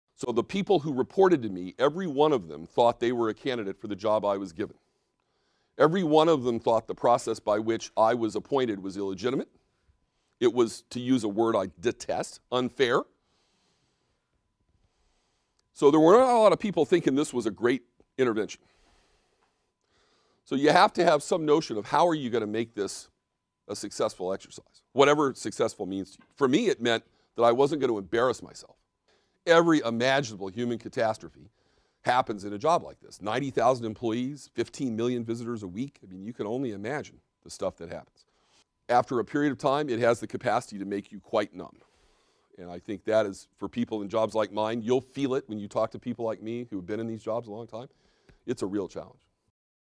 Gary Loveman: Paths to Power
Gary Loveman, a former assistant professor at Harvard Graduate School of Business Administration, became Chief Operating Officer of Harrah's Entertainment in 1998. He was an outsider in a very insular business who had no significant administrative or managerial experience.

In this video, Loveman discusses his definition of success, his strategy for achieving it, and how everyone who reported to him believed that he or she was a better candidate for the COO position than Loveman was.  He talks about critical relationships and how to serve those relationships on a daily basis so that key people do not feel marginalized or cut out.  Loveman also explains that as one moves into senior positions, he or she no longer has the luxury of relationships not working.  Instead, senior executives are responsible for making critical relationships work so that the company is well served.